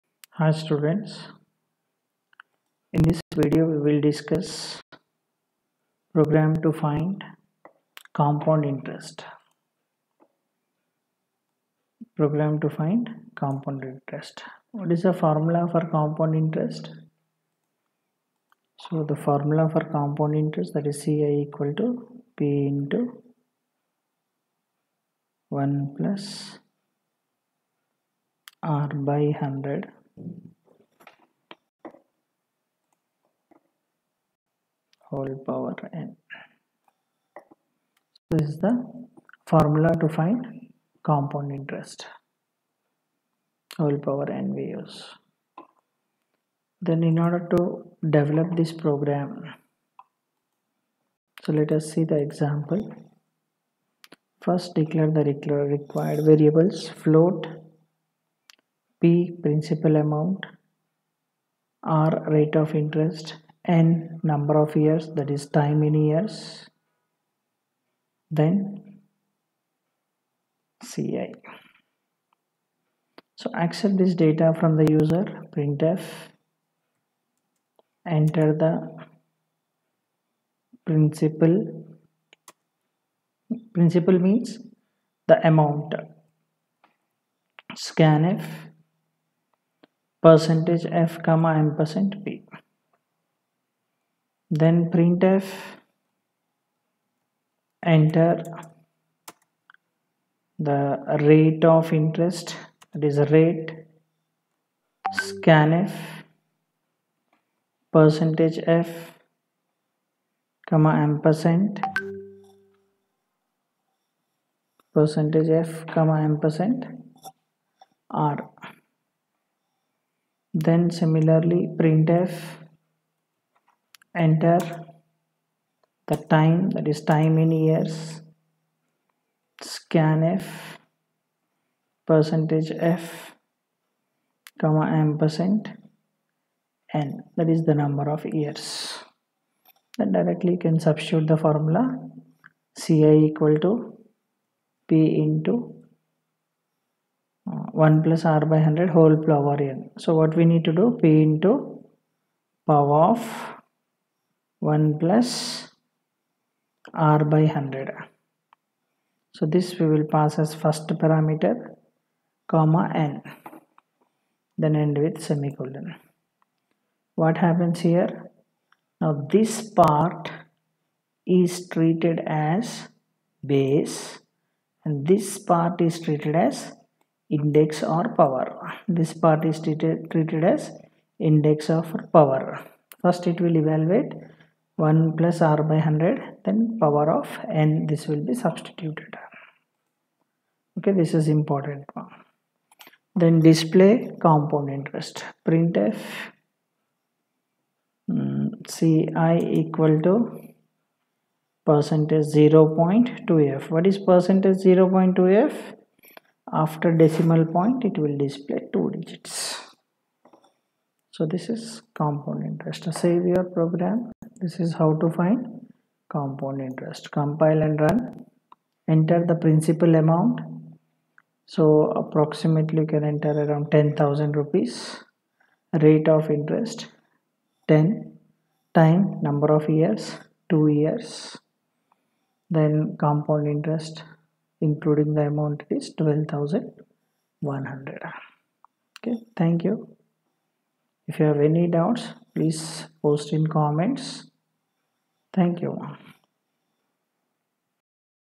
C2.10 program to find Compound Interest | C Language | for Beginners
This video helps you to find Compound Interest